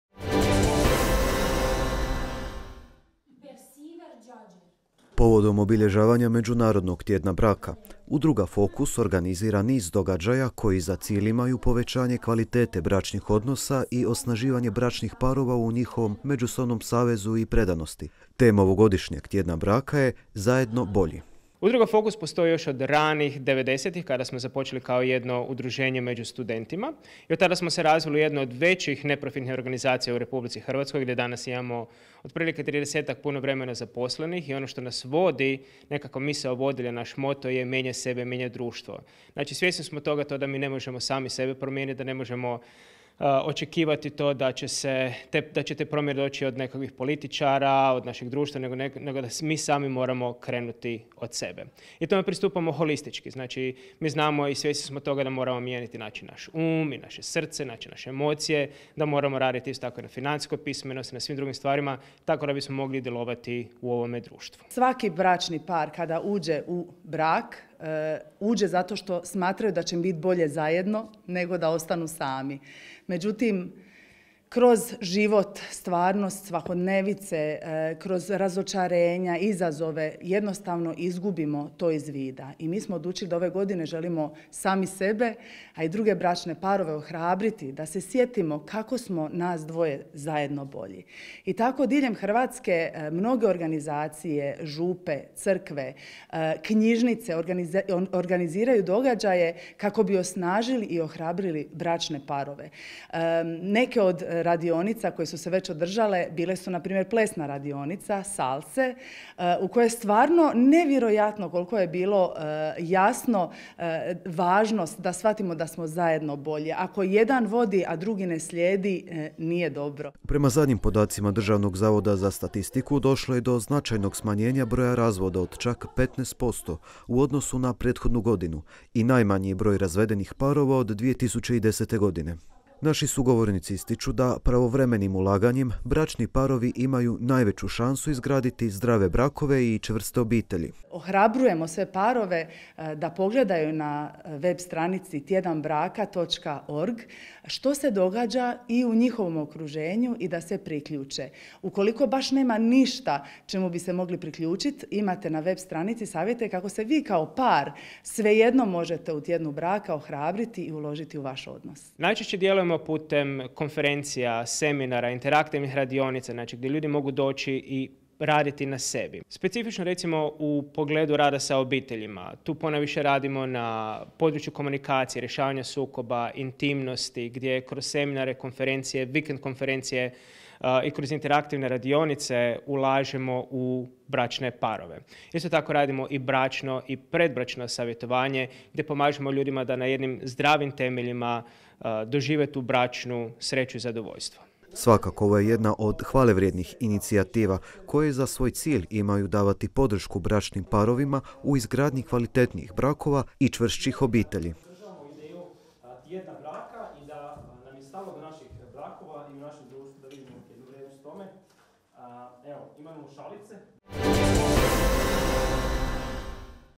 Pregled dana - Tjedna braka: “Zajedno bolji”
Povodom obilježavanja Međunarodnog tjedna braka Udruga Fokus organizira niz događaja koji za cilj imaju povećanje kvalitete bračnih odnosa i osnaživanje bračnih parova u njihovu međusobnom savezu i predanosti, te njihov utjecaj na dobrobit našeg društva. Tema ovogodišnjeg Tjedna braka je “Zajedno bolji”.